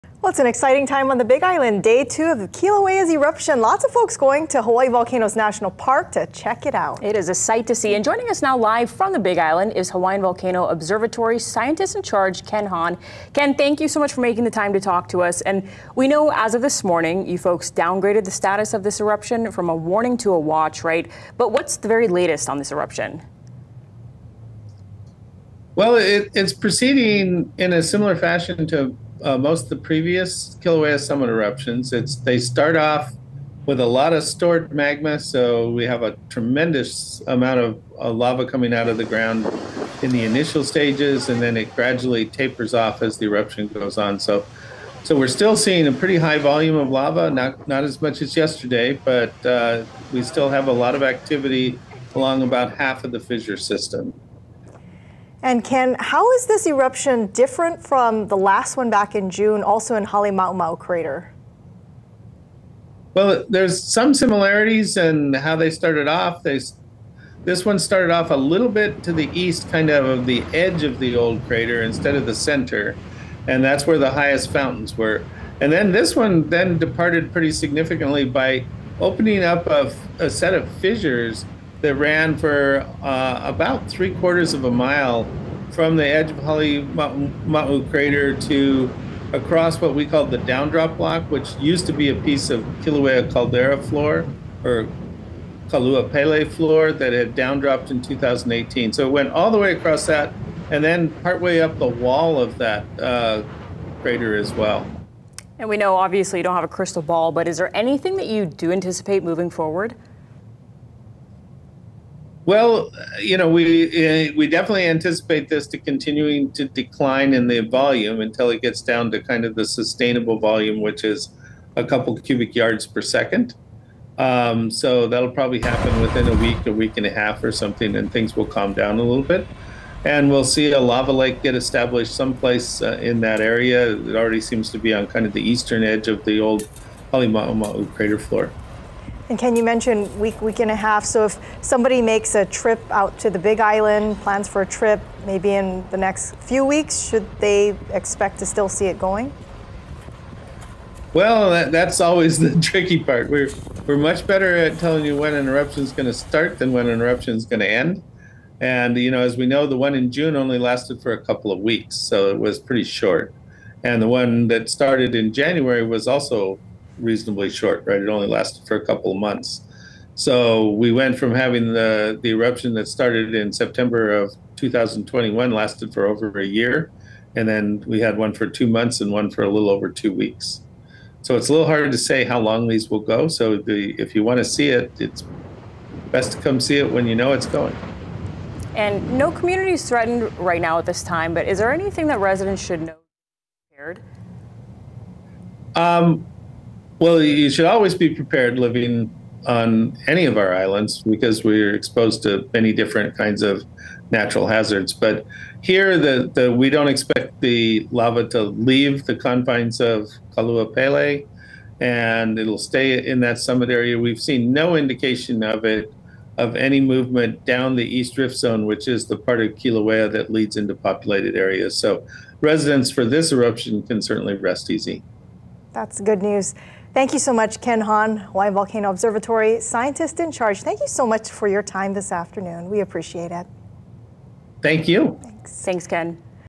Hawaii Volcano Observatory scientist Ken Hon talks about latest Kilauea eruption
It's day two of Kilauea's eruption, with hundreds of tourists going to Hawaii Volcanoes National Park to check it out. Hawaiian Volcano Observatory Scientist in Charge Ken Hon joined Island News to talk about this latest eruption.